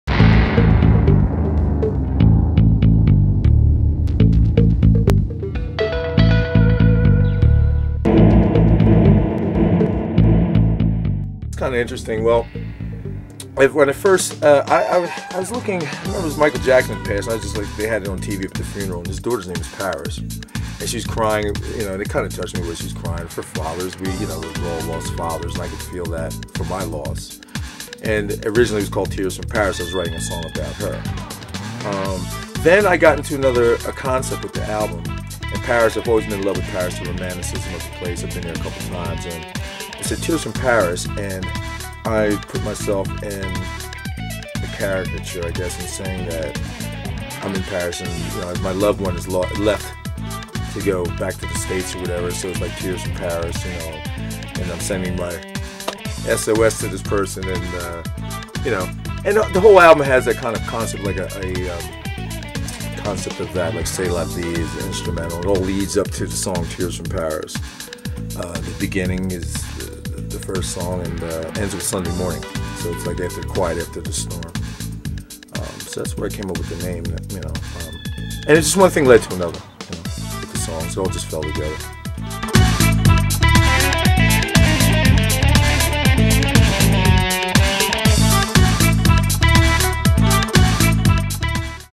Jeff Washington - CD promo
Jeff talks about his new CD release.
A George Manney film.